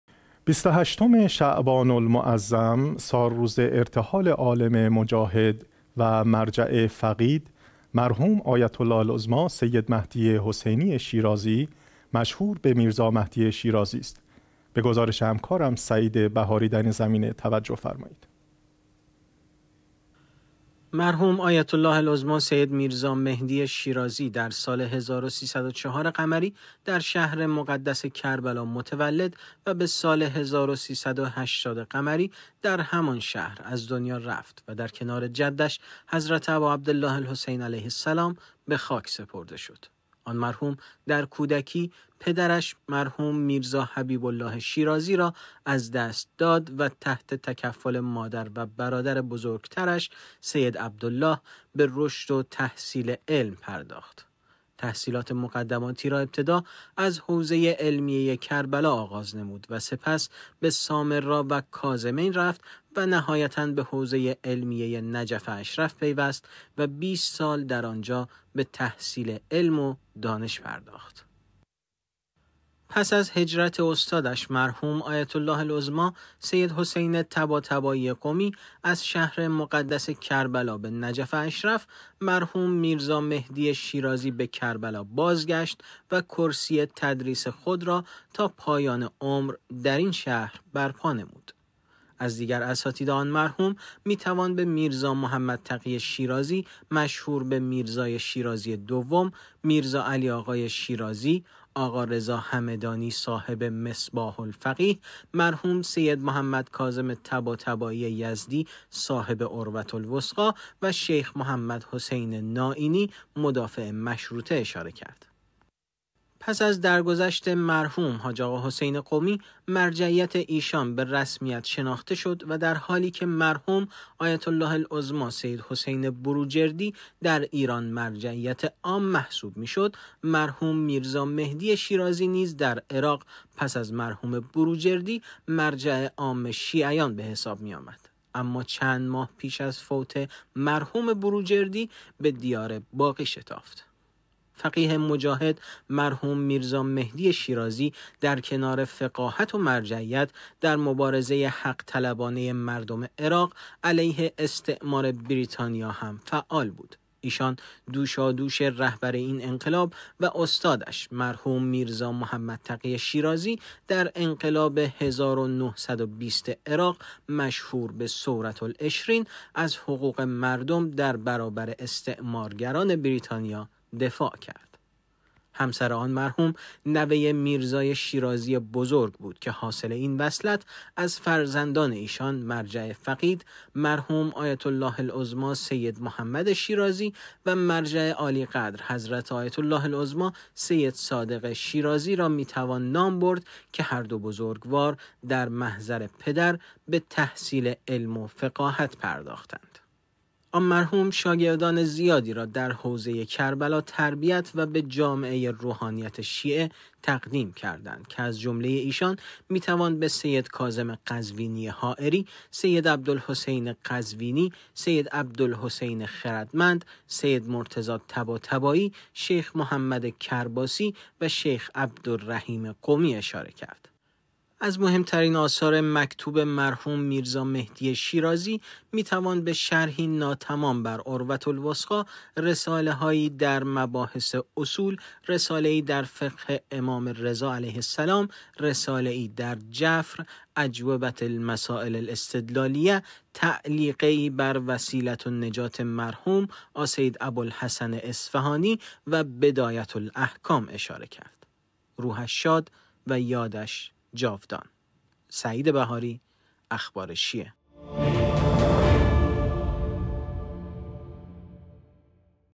سالروز ارتحال فقیه مجاهد، میرزا مهدی شیرازی ،به گزارش سعید بهاری در این زمینه توجه فرمایید
28 شعبان المعظم سالروز ارتحال عالم مجاهد و مرجع فقید مرحوم آیت الله العظمی سید مهدی حسینی شیرازی مشهور به میرزا مهدی شیرازی است.
مرحوم آیت الله العظمی سید میرزا مهدی شیرازی در سال 1304 ق در شهر مقدس کربلا متولد و به‌سال 1380 ق در همان شهر از دنیا رفت و در کنار جدش حضرت اباعبدالله الحسین علیه السلام به خاک سپرده شد.
آن مرحوم در کودکی، پدرش مرحوم میرزا حبیب الله شیرازی را از دست داد و تحت تکفل مادر و برادر بزرگترش سید عبدالله به رشد و تحصیل علم پرداخت. تحصیلات مقدماتی را ابتدا از حوزه علمیه کربلا آغاز نمود و سپس به سامرا و کاظمین رفت و نهایتا به حوزه علمیه نجف اشرف پیوست و بیست سال در آنجا به تحصیل علم و دانش پرداخت.
پس از هجرت استادش مرحوم آیت الله العظمی سید حسین طباطبایی قمی از شهر مقدس کربلا به نجف اشرف، مرحوم میرزا مهدی شیرازی به کربلا بازگشت و کرسی تدریس خود را تا پایان عمر در این شهر برپا نمود.
از دیگر اساتید آن مرحوم می توان به میرزا محمد تقی شیرازی مشهور به میرزای شیرازی دوم، میرزا علی آقا شیرازی، آقا رضا همدانی صاحب مصباح الفقیه، مرحوم سید محمد کاظم طباطبایی یزدی صاحب عروة الوثقى، و شیخ محمد حسین نائینی مدافع مشروطه اشاره کرد.
پس از درگذشت مرحوم حاج آقا حسین قمی، مرجعیت ایشان به رسمیت شناخته شد و در حالیکه مرحوم آیت الله العظمی سید حسین بروجردی در ایران مرجعیت عام محسوب می‌شد مرحوم میرزا مهدی شیرازی نیز در عراق پس از مرحوم بروجردی مرجع عام شیعیان به‌حساب می آمد اما چند ماه پیش از فوت مرحوم بروجردی به دیار باقی شتافت.
فقیه مجاهد مرحوم میرزا مهدی شیرازی در کنار فقاهت و مرجعیت در مبارزه حق طلبانه مردم عراق علیه استعمار بریتانیا هم فعال بود. ایشان دوشادوش رهبر این انقلاب و استادش مرحوم میرزا محمد تقی شیرازی در انقلاب 1920 عراق مشهور به ثورة العشرين از حقوق مردم در برابر استعمارگران بریتانیا دفاع کرد.
همسر آن مرحوم نوه میرزای شیرازی بزرگ بود که حاصل این وصلت از فرزندان ایشان، مرجع فقید مرحوم آیت الله العظمی سید محمد شیرازی و مرجع عالیقدر حضرت آیت الله العظمی سید صادق شیرازی را می توان نام برد که هر دو بزرگوار در محضر پدر به تحصیل علم و فقاهت پرداختند.
آن مرحوم شاگردان زیادی را در حوزه کربلا تربیت و به جامعه روحانیت شیعه تقدیم کردند که از جمله ایشان می‌توان به سید کاظم قزوینی حائری، سید عبدالحسین قزوینی، سید عبدالحسین خردمند، سید مرتضی طباطبایی، شیخ محمد کرباسی و شیخ عبدالرحیم قمی اشاره کرد.
از مهمترین آثار مکتوب مرحوم میرزا مهدی شیرازی می توان به شرحی ناتمام بر عروة الوثقی ، رساله هایی در مباحث اصول ، رساله ای در فقه امام رضا علیه السلام ، رساله ای در جفر ، اجوبه المسائل الاستدلالیه ، تعلیقه‌ای بر وسیله النجات مرحوم آسید ابوالحسن اصفهانی و بدایه الاحکام اشاره کرد.
روحش شاد و یادش جاودان.